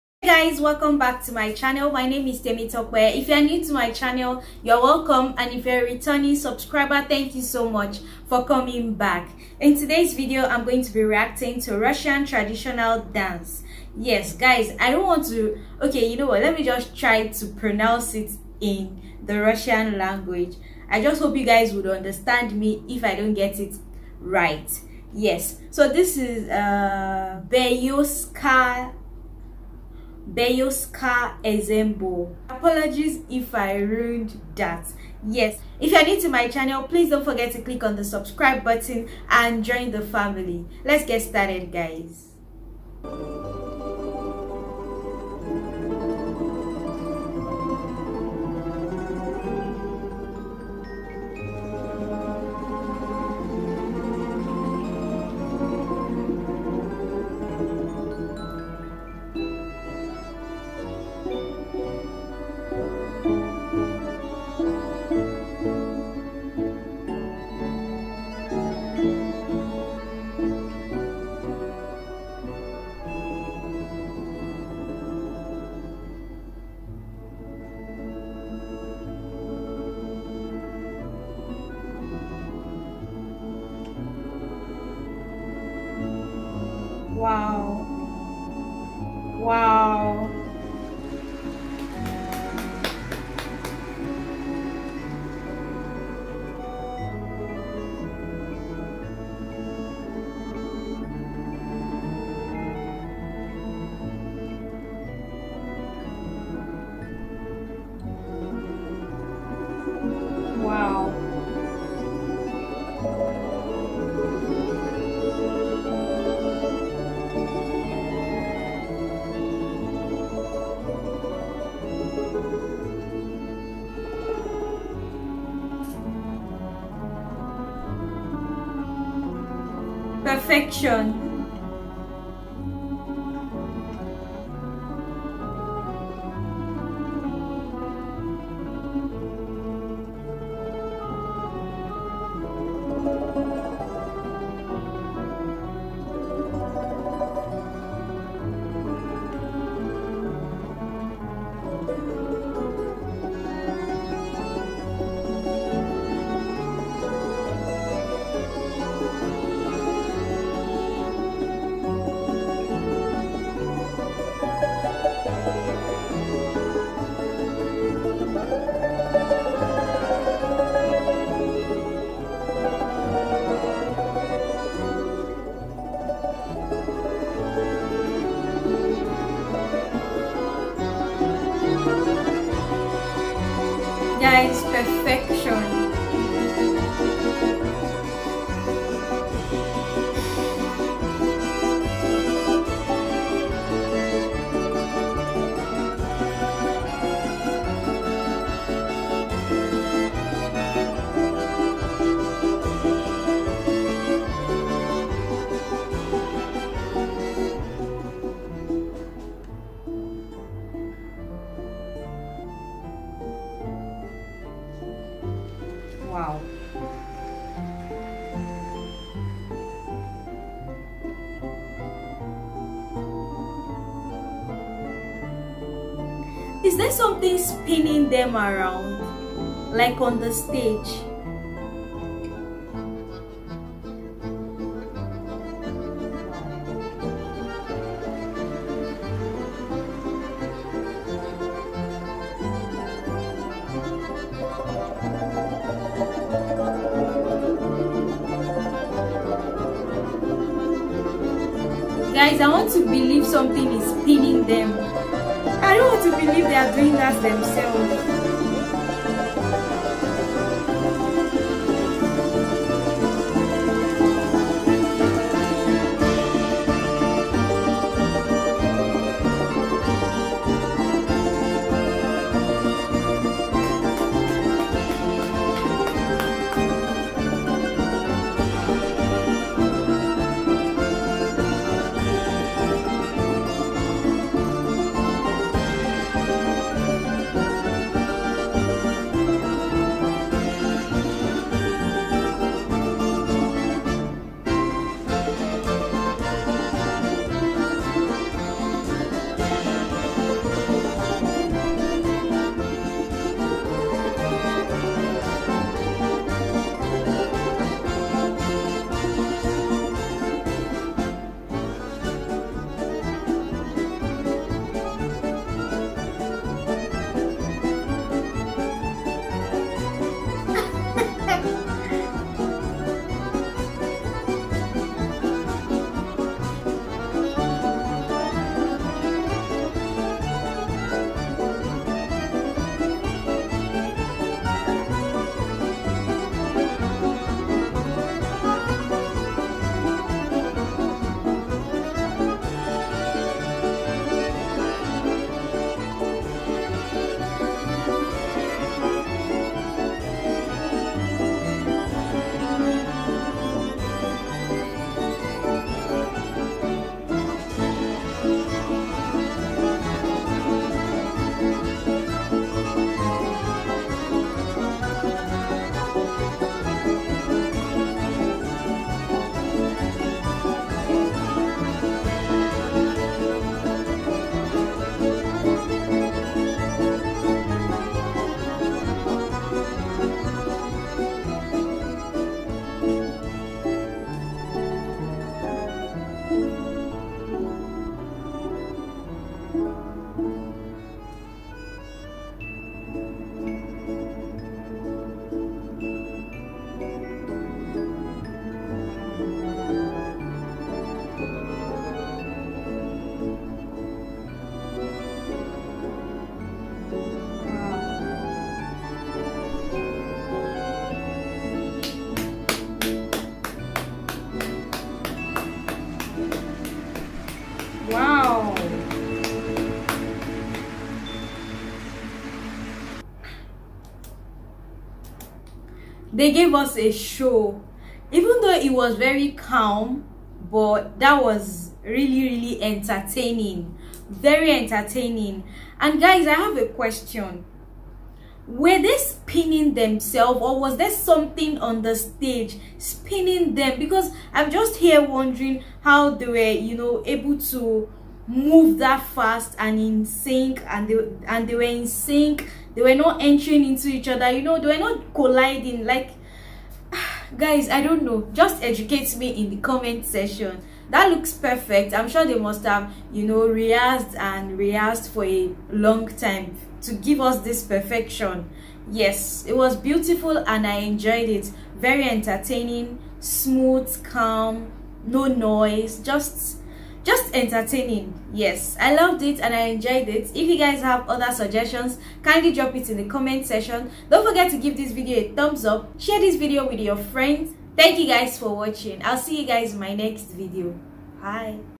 Russian Traditional Dance | Beryozka Ensemble Folk Dance REACTION
A playlist of my reactions to EVERYTHING RUSSIA (You should check this out)😊